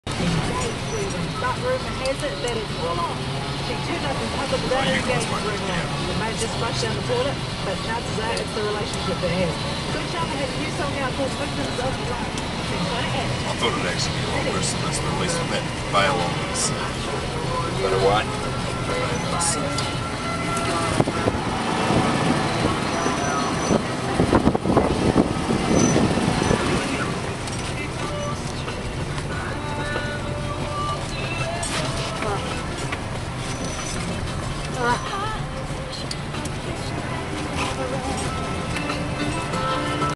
extreme trucking new zealand
a dirty little track at lambhill, dunedin and take my word its was worse that it looks